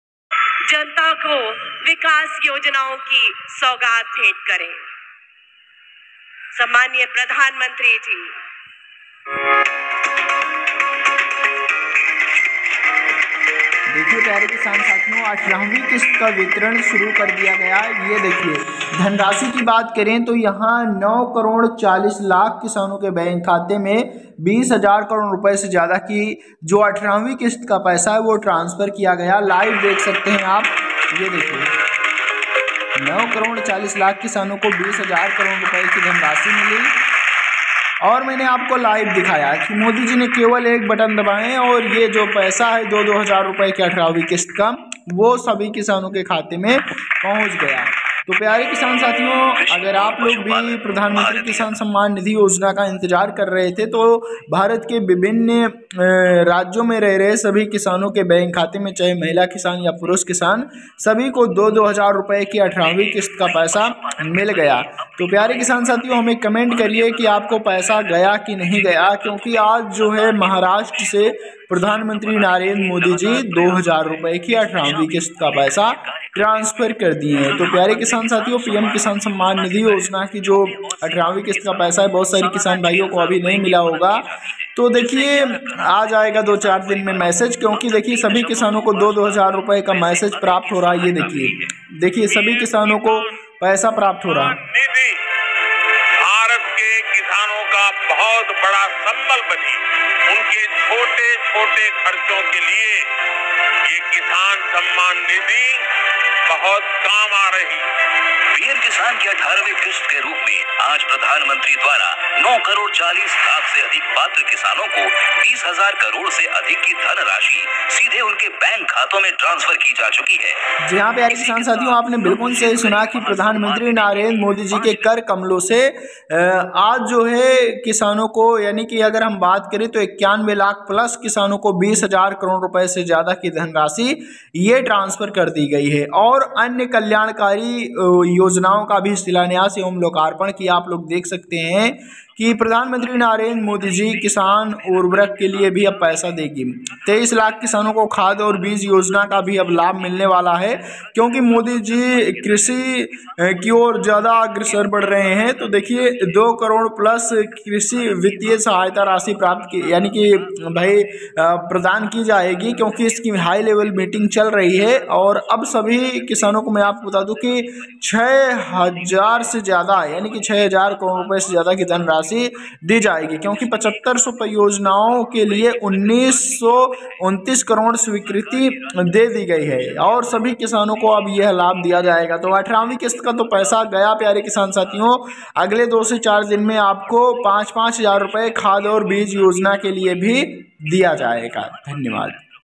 अक्टूबर 2024 को महाराष्ट्र के वाशिम से मोदी जी ₹2000 की 18वीं किस्त का वितरण कर रहे हैं।
आज 6 अक्टूबर 2024 को महाराष्ट्र के वाशिम से मोदी जी ₹2000 की 18वीं किस्त का वितरण कर रहे हैं।